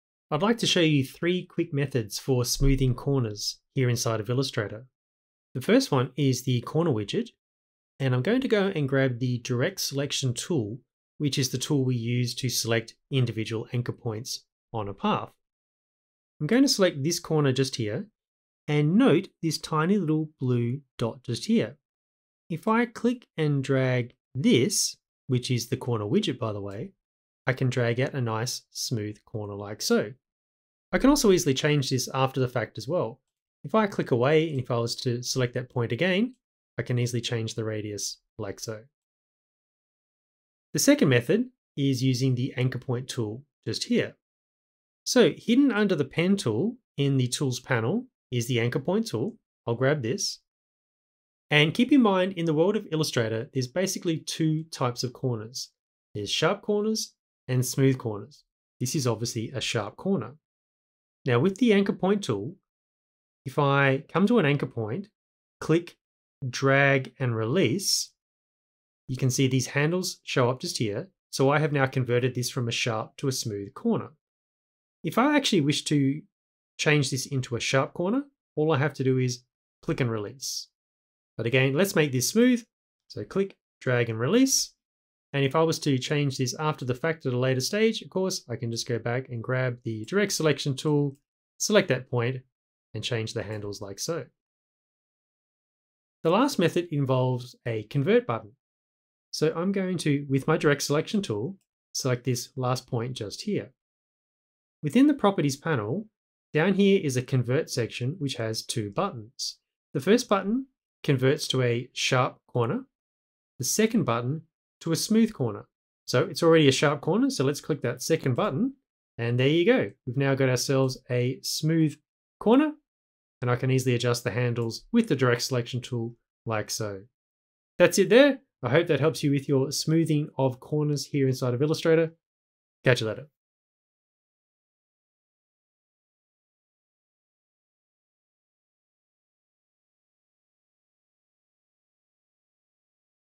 How to Smooth Corners in Illustrator
Learn 3 quick methods to convert a sharp corner into a smooth corner in Illustrator. Details below...

0:00 - Corner Widget
When a path or individual anchor points are selected and sharp corners are included, those corners will show a blue dot surrounded by a ring just inside the corner. When that Corner Widget is dragged it will round out the corner. This is a non-destructive process that can be easily adjusted after the fact.

0:40 - Anchor Point Tool
This tool can be found under the Pen tool within the Tools panel (keyboard shortcut: Shift+C). It is used to convert anchor points between sharp and smooth corners. To convert a sharp corner, click, drag and release on the anchor point to drag out handles. To convert a smooth corner, simply click and release to remove the handles. The handles on smooth corners can be adjusted at any time using the Direct Selection Tool.

1:35 - Convert Button
When an individual anchor point is selected, the Properties panel will display a Convert section that contains two buttons. The first button will convert to a sharp corner while the second button will convert to a smooth corner.